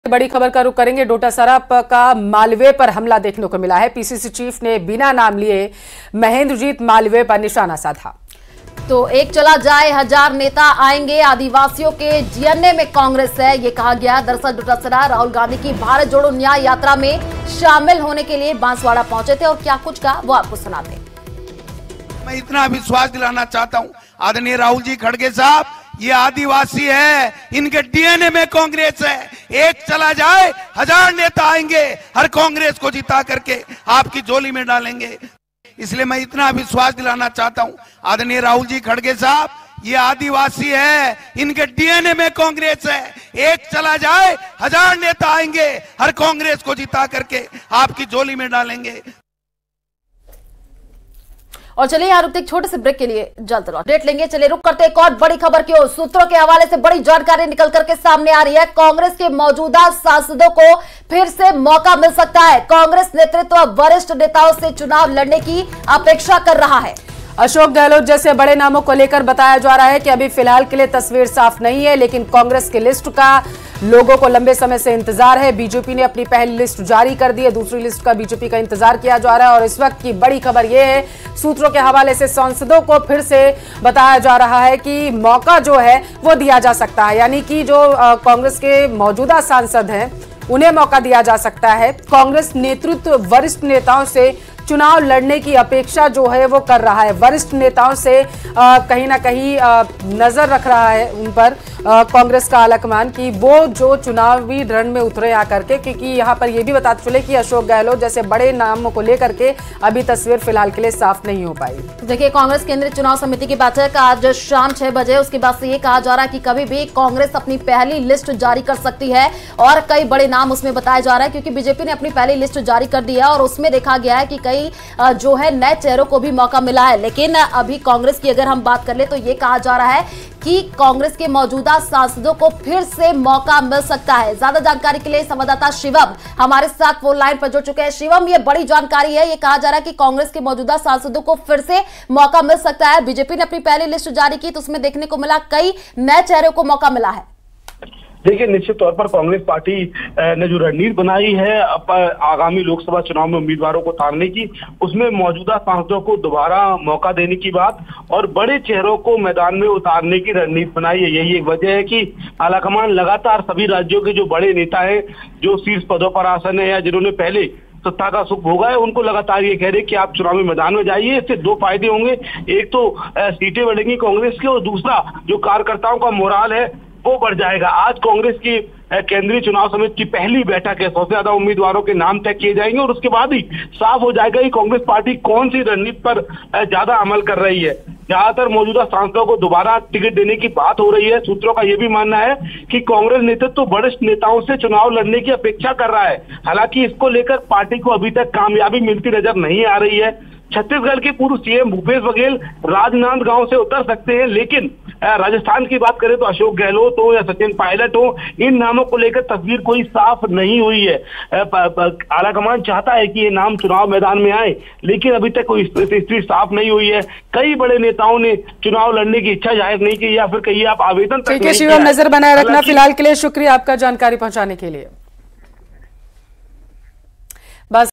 Rajasthan News : एक चला जाए, हजार नेता आएंगे- Govind Dotasra | Bharat Jodo Nyay Yatra | Rahul Gandhi
पूरे देश में Rahul Gandhi की Bharat Jodo Nyay Yatra जा रही है. इसी बीच आज Nyay Yatra, Rajasthan के Banswara में प्रवेश किया है ...